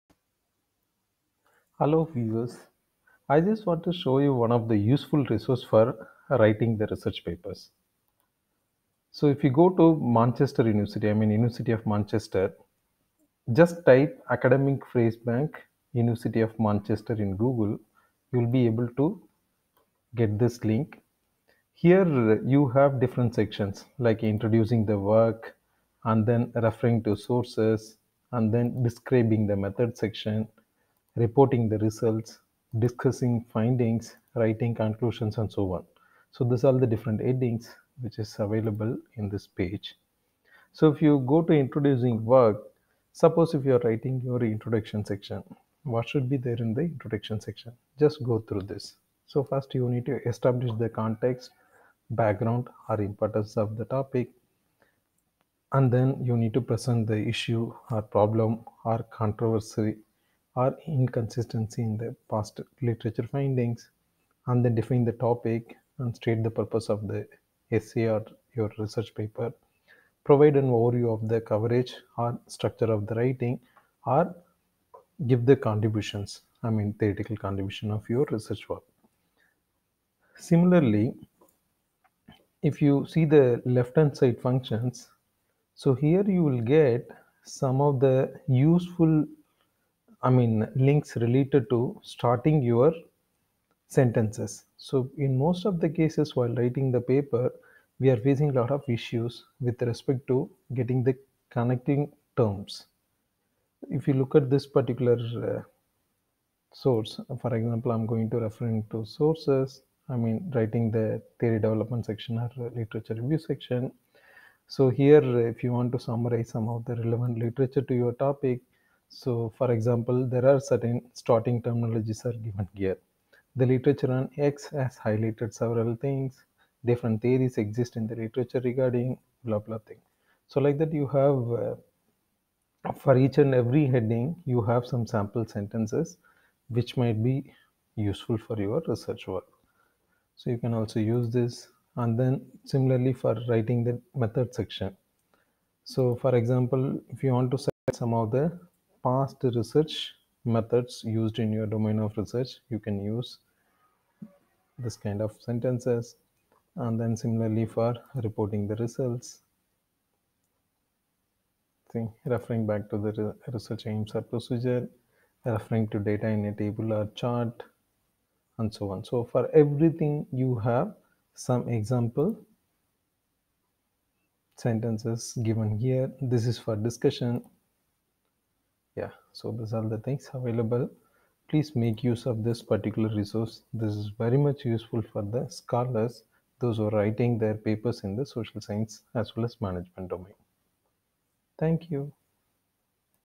Academic Phrasebank
An useful resource for academic writing in social science and management research.